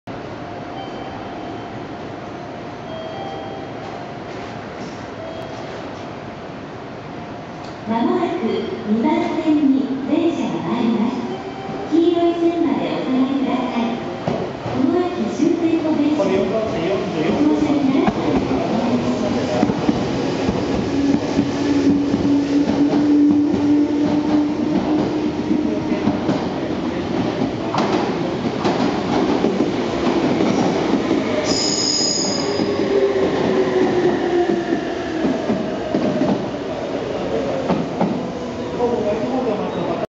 名古屋鉄道６８００系普通「中部国際空港行き」金山駅発車
名古屋鉄道６８００系(後期型)　普通　常滑線を金山駅で。